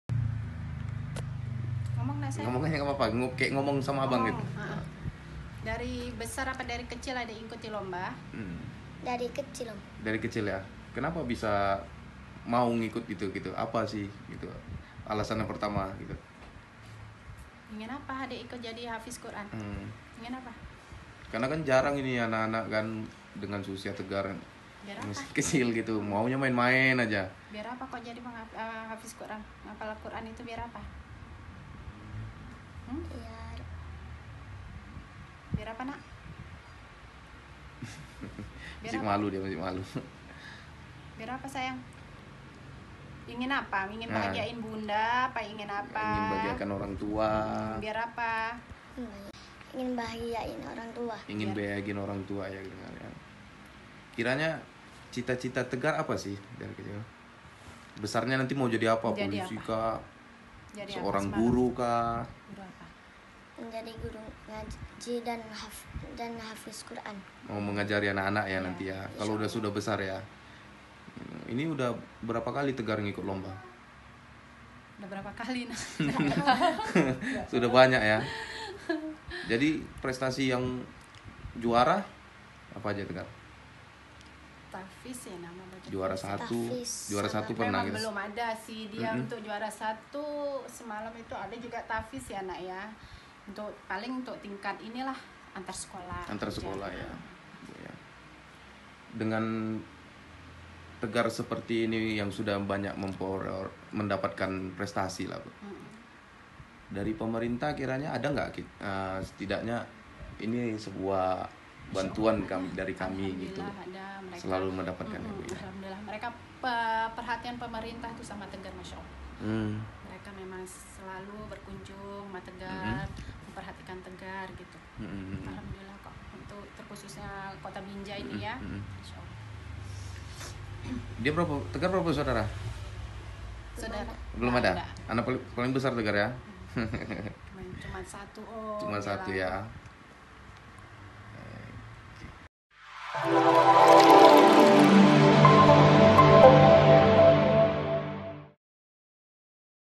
Hafal Puluhan Surah Alquran, Tegar Penyandang Disabilitas Bercita cita Menjadi Guru Ngaji
TRIBUN MEDAN.COM, BINJAI - Penyandang disabilitas, Muhammad Tegar Setiawan, bercita-cita ingin menjadi seorang guru ngaji. Tegar, sapaan akrabnya, adalah bocah yang sudah berulangkali mendapatkan penghargaan, berkat kemahirannya menghafal Al-quran.

Diusia 8 tahun, saat ini dirinya sudah bisa menghafal 6 juz Alquran.

"Cita-cita saya ingin menjadi seorang guru ngaji, agar bisa mengajarkan anak-anak nantinya, untuk bisa membaca ayat suci Al-Quran," kata dia, saat ditemui, di rumahnya Jalan Danau Tondano, Lingkungan IX, Kelurahan Sumber Karya, Kecamatan Binjai Timur, Senin (26/4/20201).

Walau tidak seperti anak pada umumnya, Tegar, memiliki sifat dan cara berpikir seusianya. Dirinya, saat ditemui masih malu-malu, untuk bercerita dengan orang yang baru ditemui.

Dengan wajah polosnya, ia bercerita mengapa memilih sebagai Tahfiz Al-Quran, karena tidak mau dipandang sebelah mata oleh orang lain. Kemampuan Tegar melafazkan hapalan puluhan Surat Al Quran sontak menjadi menarik perhatian masyarakat.